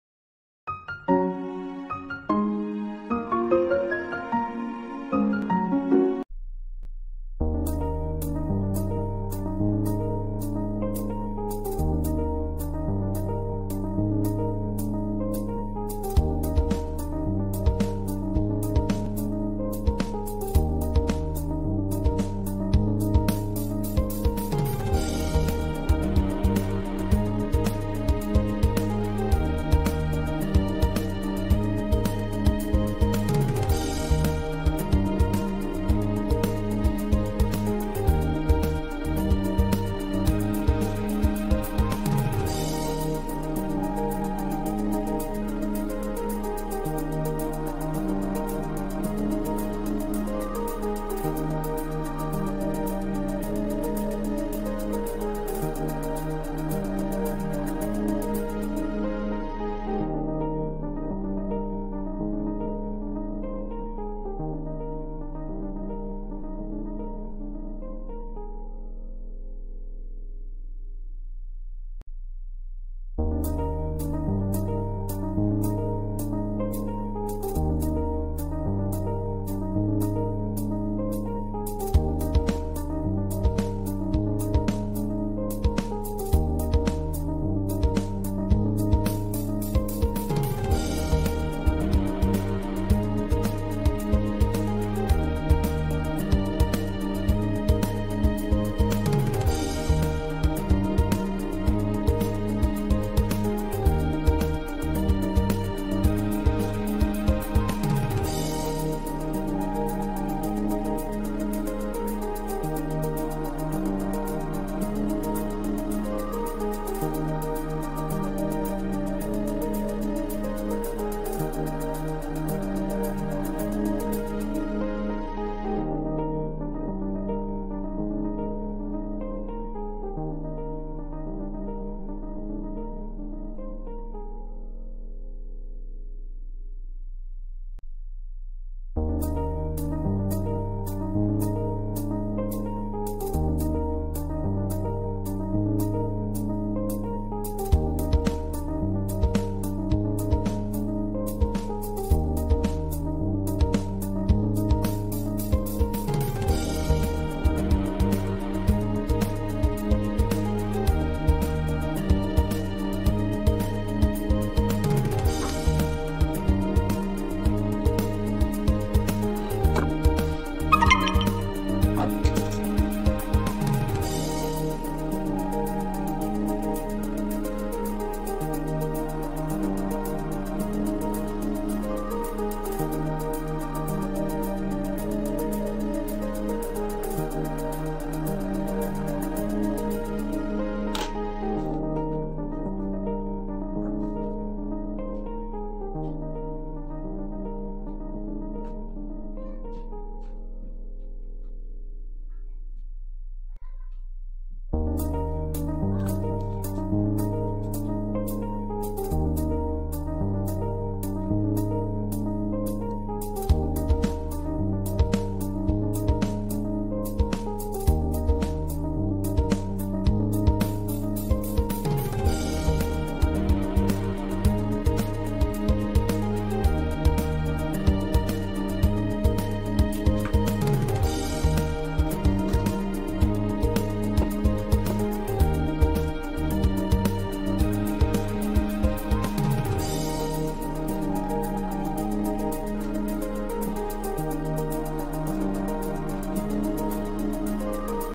Unicorn 🦄 shapers box with waste box 🎁 🤩
If you like it  plss

Material Required -
💕 Waste box
💕glitter
💕 glue
💕 Card board
💕Paper